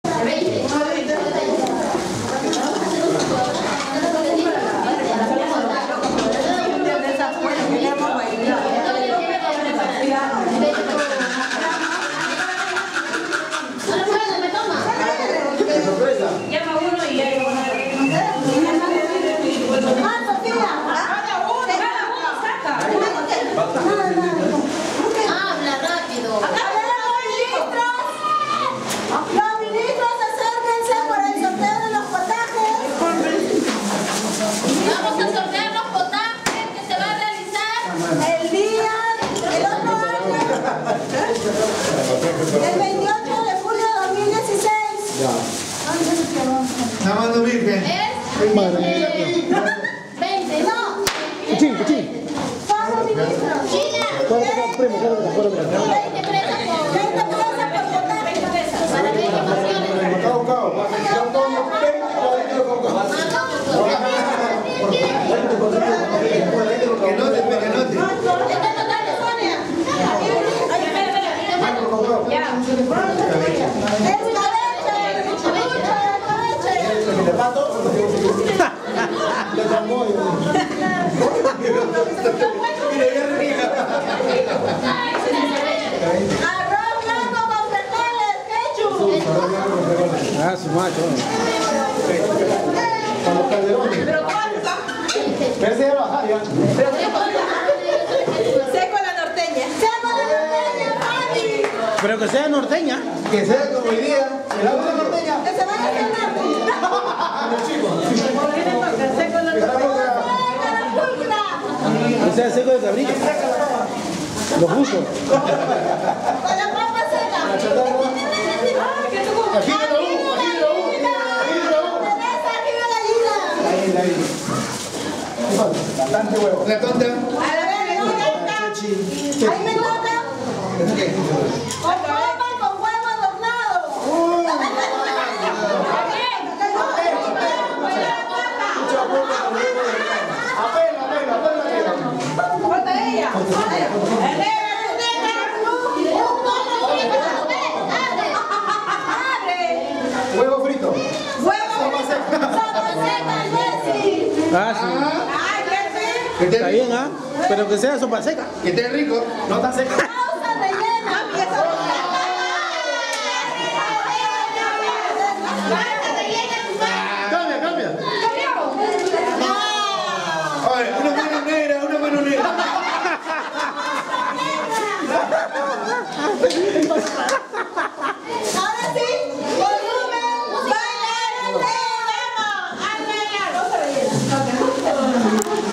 Sorteo de Viandas para el 28 de Julio 2016
Aqui los Ministros anunciando sus viandas para el 28 de Julio del 2016, a prepararse...